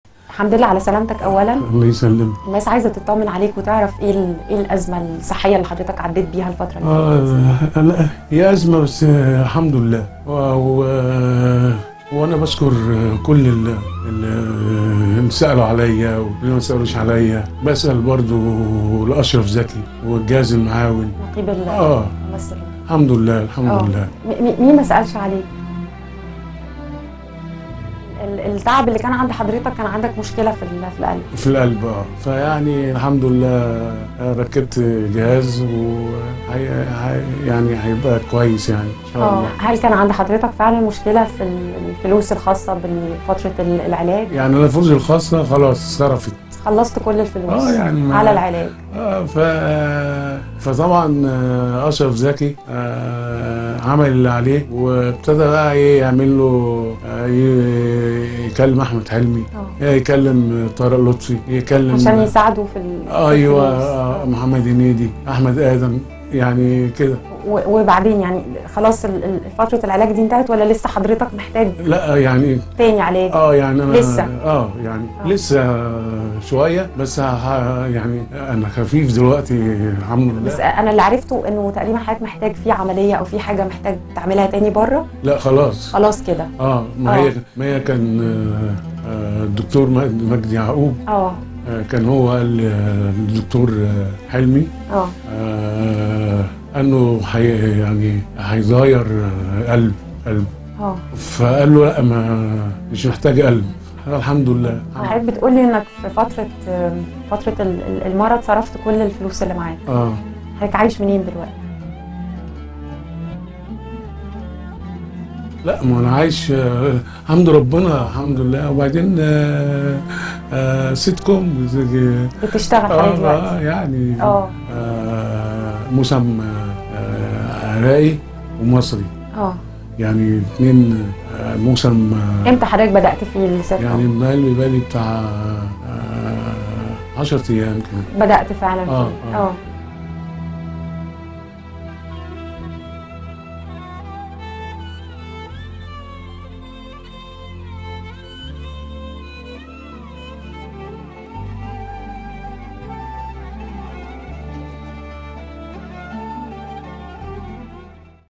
فلوسي كلها صرفتها على العلاج ومعيش فلوس أجيب علاج💔 " قبل وفاتة "
تابعوا قناة DSD 1  على

إعداد وتقديم
أحمد هاني

🔴 لا تنسى الإشتراك في القناة وتفعل زر الجرس 🔔 ليصلك كل جديد .